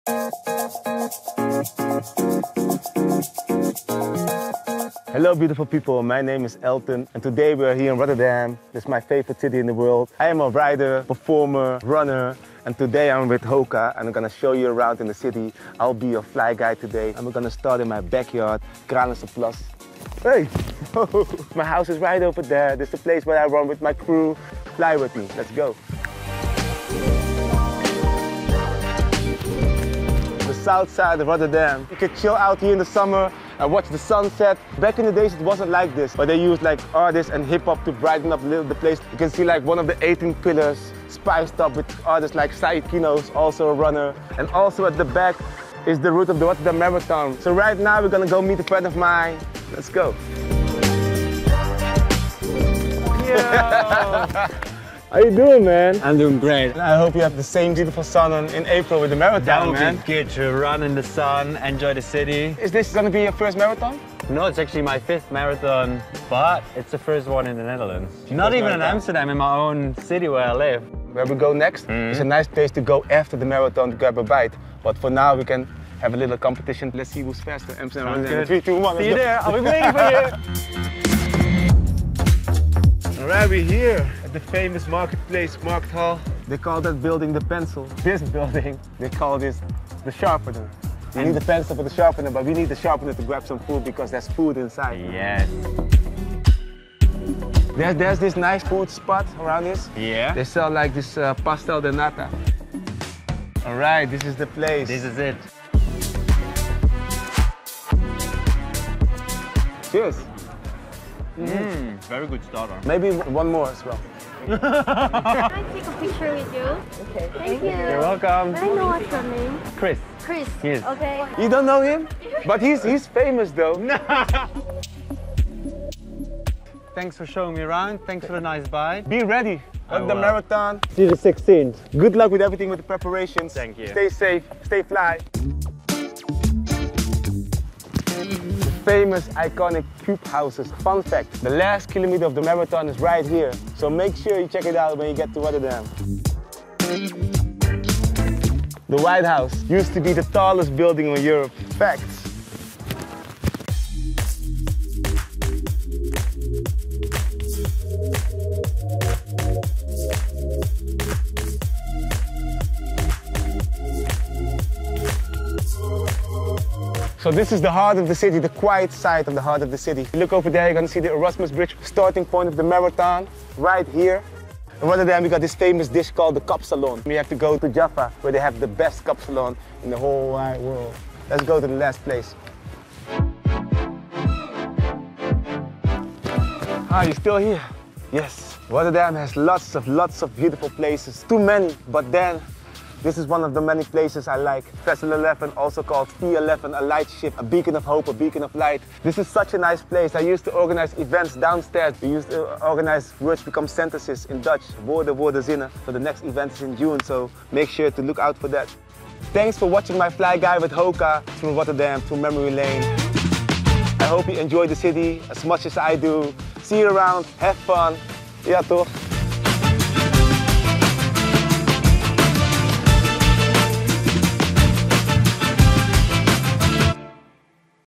Fly Guide: Rotterdam
Our Fly Guide: Rotterdam film featuring spoken word artist Elten Keine is here!

In the run-up to the NN Rotterdam Marathon, we explored the city alongside Elten to experience Rotterdam at its very best – and yes, many stroopwafels and kapsalons were eaten.

Soaring through the cobbled streets we saw new and old Dutch brought to life and ran along some of the marathon's route.